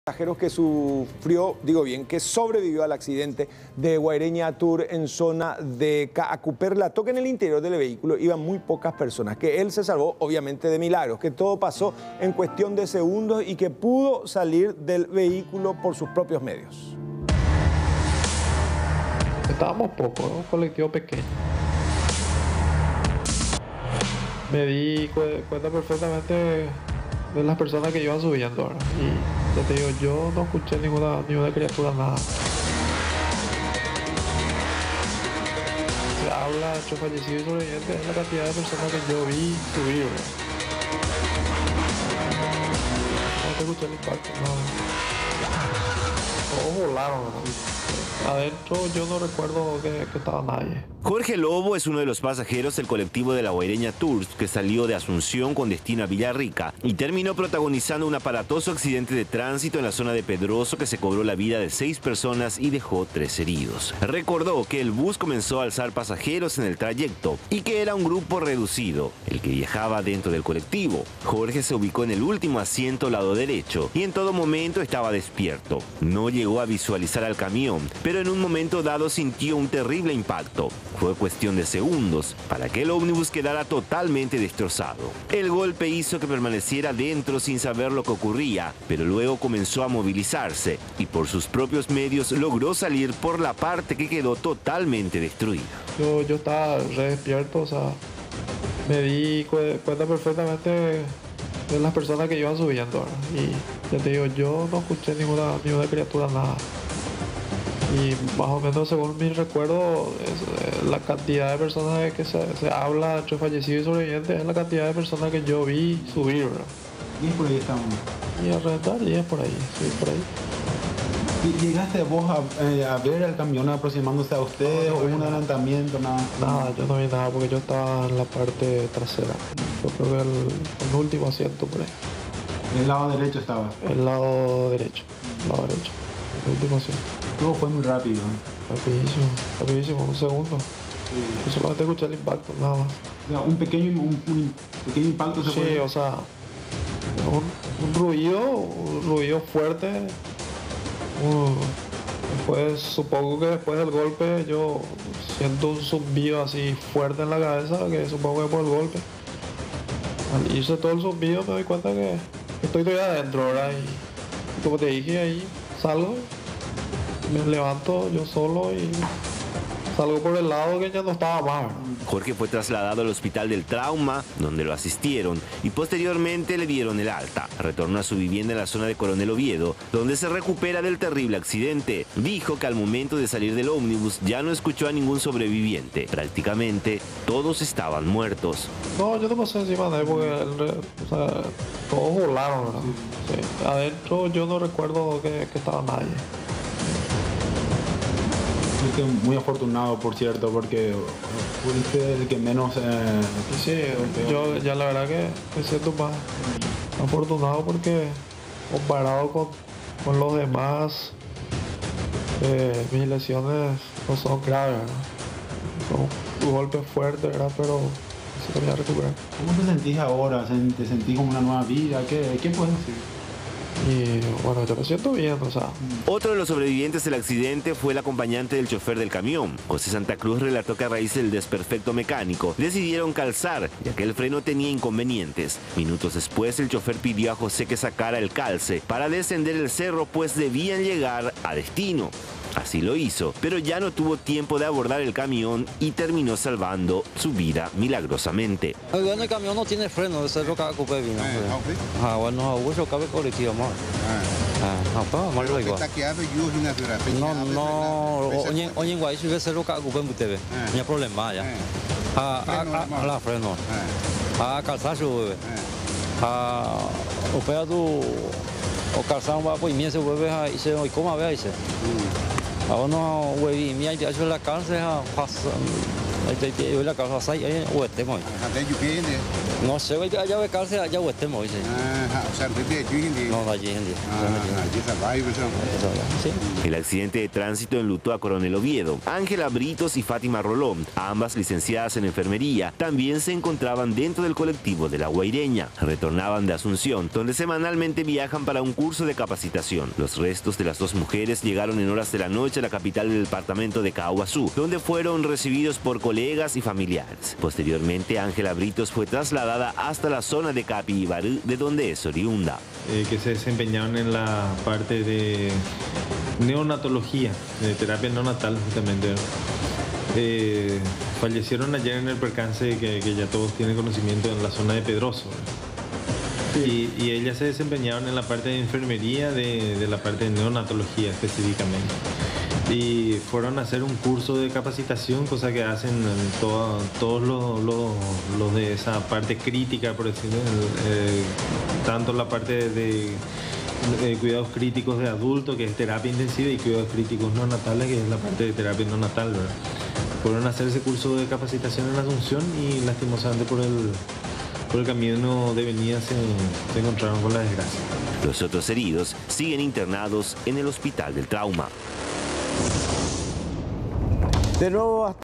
Sobreviviente de la tragedia cuenta su experiencia
Indicó que todo pasó en cuestión de segundo y que ahora él está con vida sólo de milagro, además pudo salir del vehículo por sus propios medios